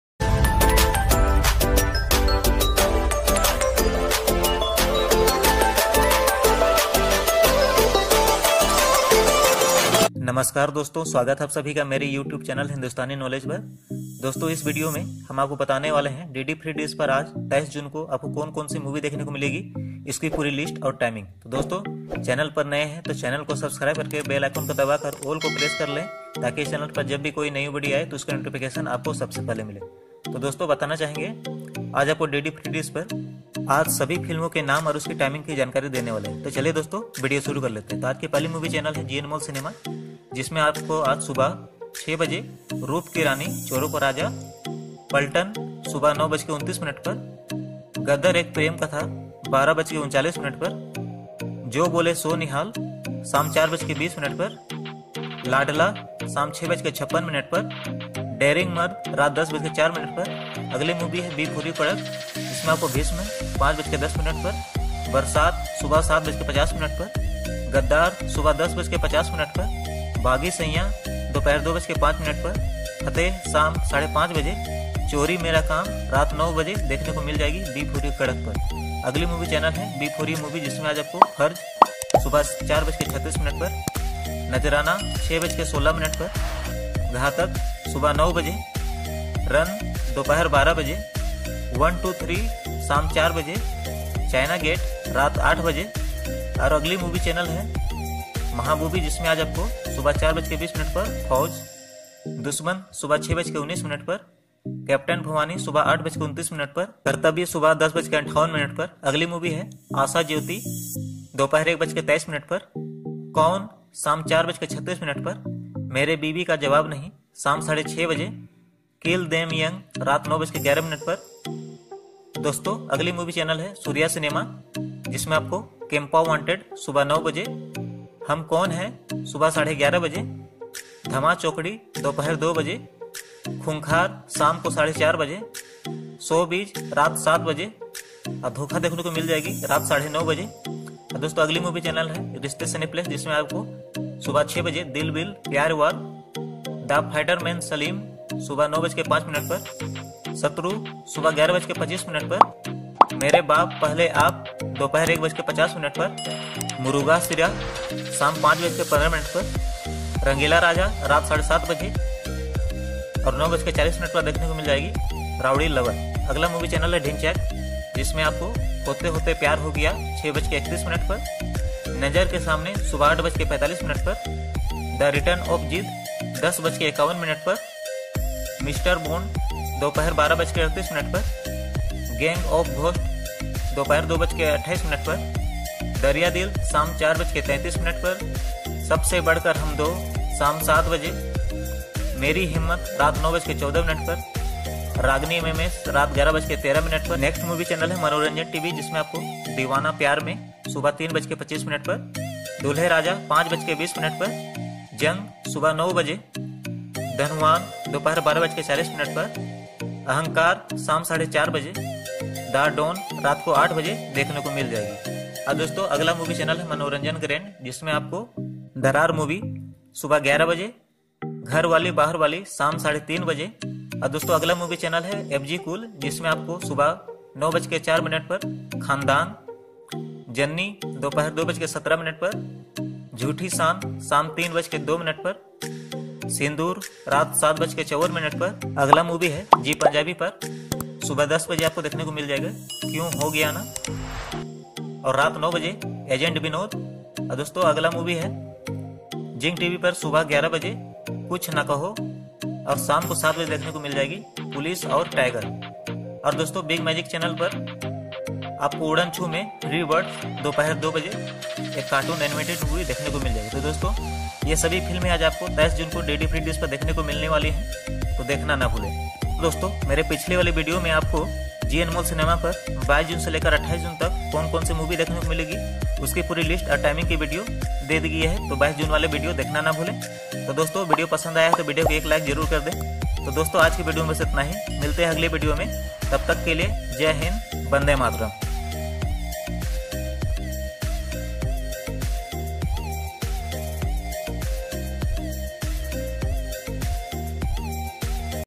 DD Free Dish All Hindi Movie Schedule List & Timing 23 June 2020 Zee Anmol cinema, Rishtey Cineplex.
Doston is video mein bataya Gaya hai DD free Dish par All Hindi movie channel schedule list and timing 23 June 2020.
Zee Anmol cinema
B4U Kadak
B4U movie
Surya cinema
Rishtey Cineplex
Dhincheck movie
Zee Punjabi
Zing TV
Maha movie
Big Magic
Enter 10 movie
ABZY Cool
Manoranjan grand
Manoranjan TV
ABZY movie...etc

DD free Dish se Judi sabhi jankari Hindustani knowledge channel per.